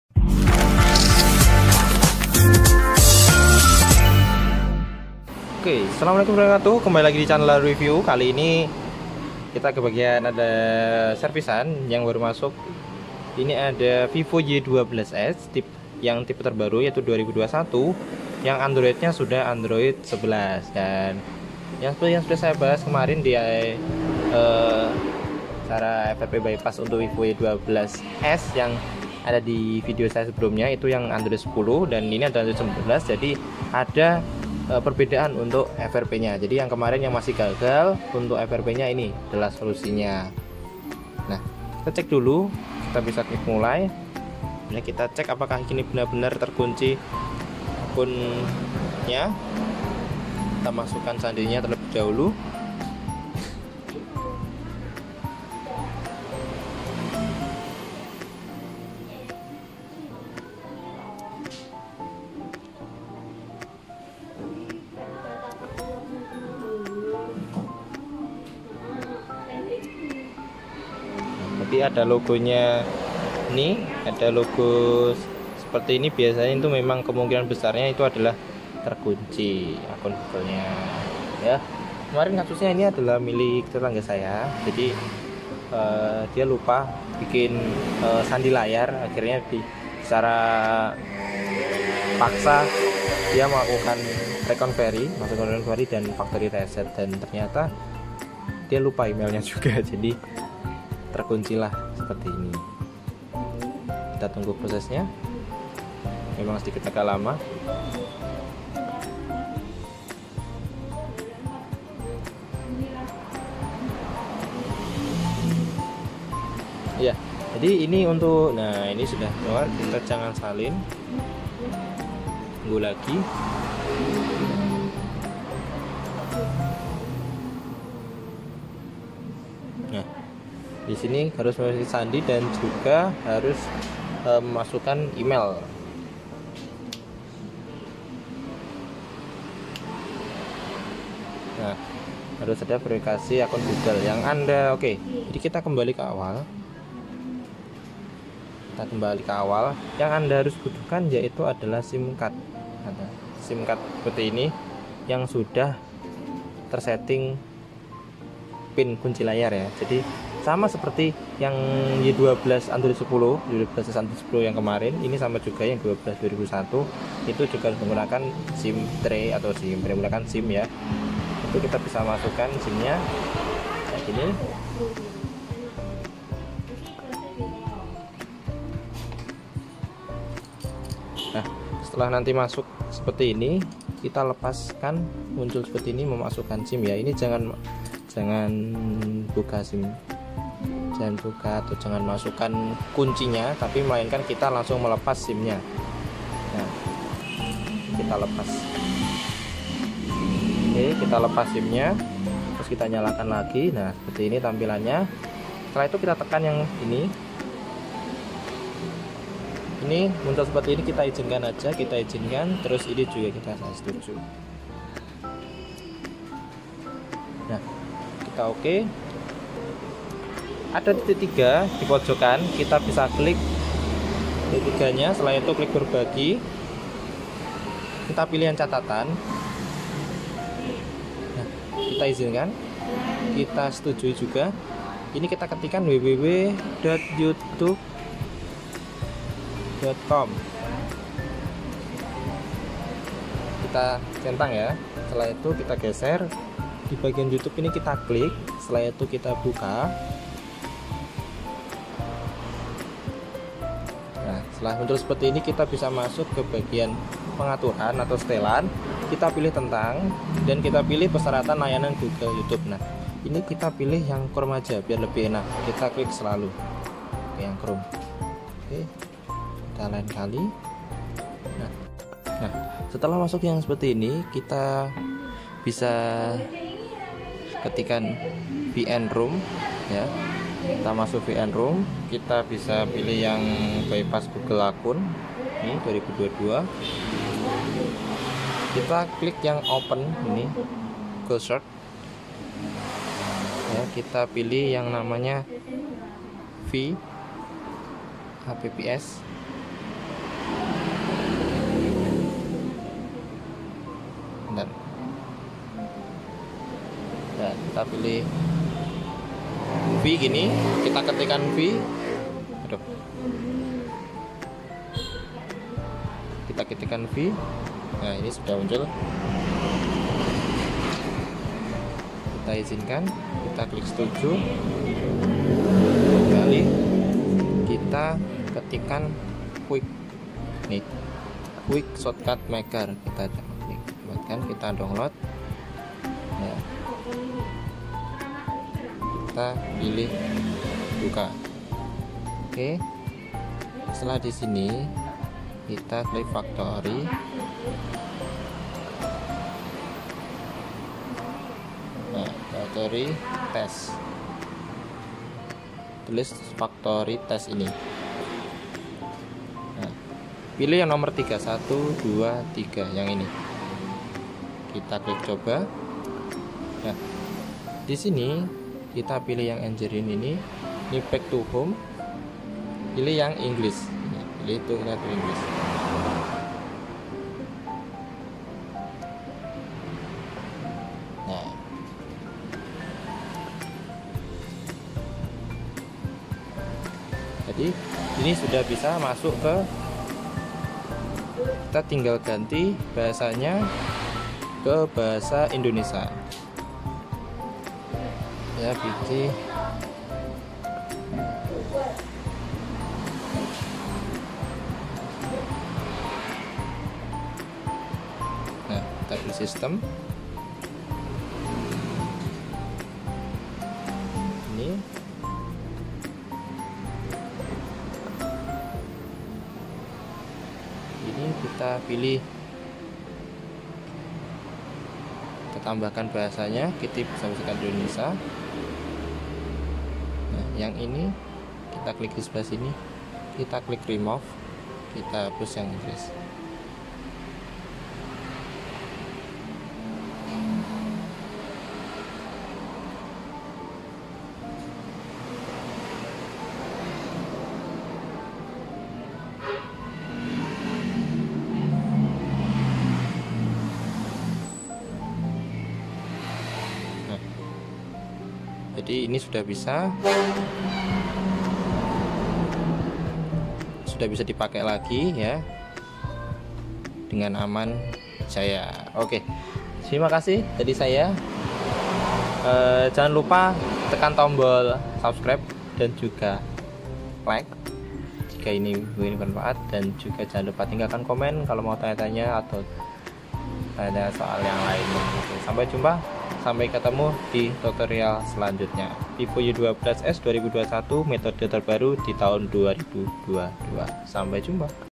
TERBARU! FRP Bypass Akun Google Vivo Y12s 2021 | Android 11 | Metode Terbaru 2022 | Tanpa PC
Hai SOBAT Review.
Kembali lagi ke sekmen service, ada servisan yg baru masuk ini ada Y12s 2021  android 11.  Berbeda dengan video yg prnah saya upload sebelumnya tntang vivo y12s lama  mengunakan android 10
.  Simak baik baik tutorialnya.